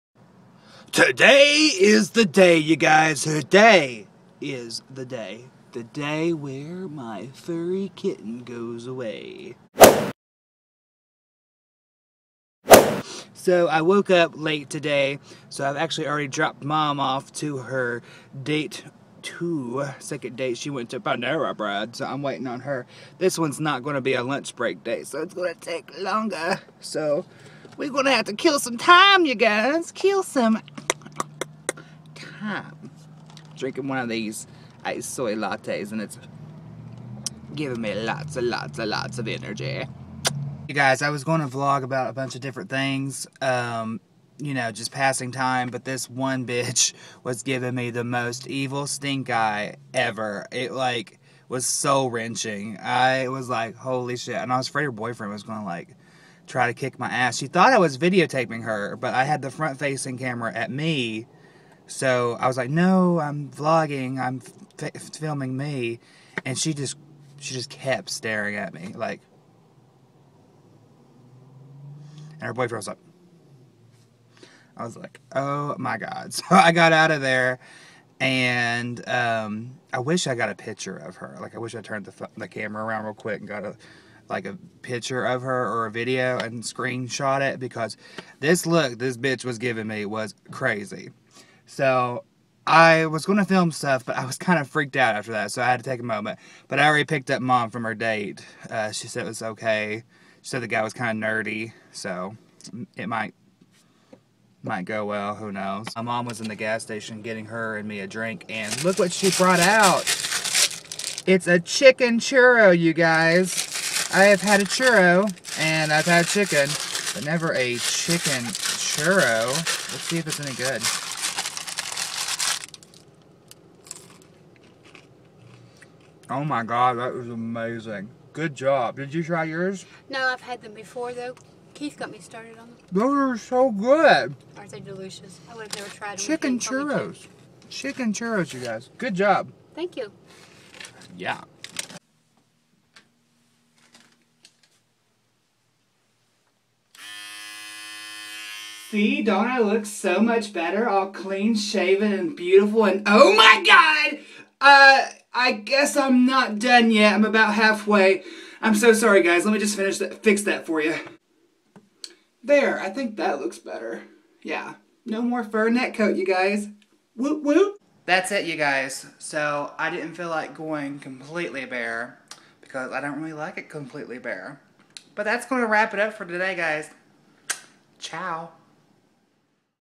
Stink Eye Chicken Churros
Public vlogging apparently can be a bit dangerous, but who cares when you have delicious chicken churros! Oh btw I tamed the fury neck beast of doom!     ...hmmph your welcome